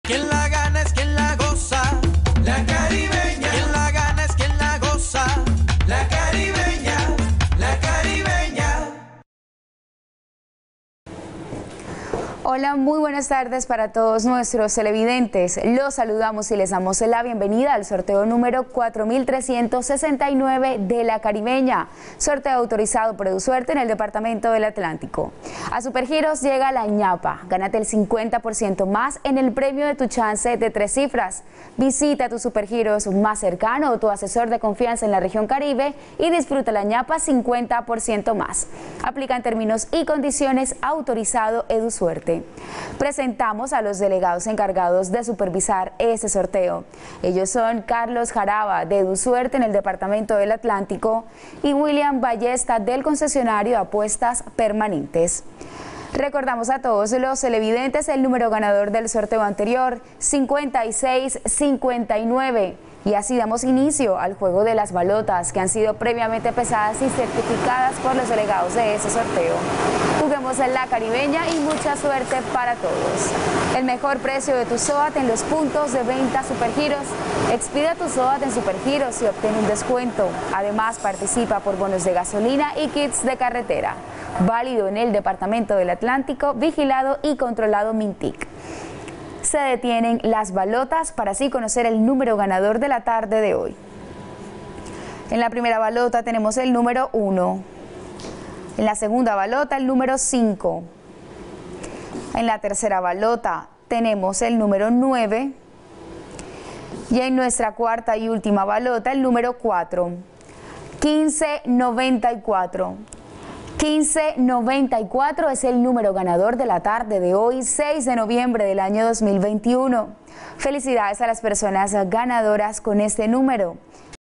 Resultado CARIBEÑA DÍA del sábado 06 de noviembre de 2021
Hola buenas tardes, te presentamos los resultados de las loterías y chances en Colombia, vamos con el Resultados del Chance Caribeña día del sábado 06 de noviembre de 2021.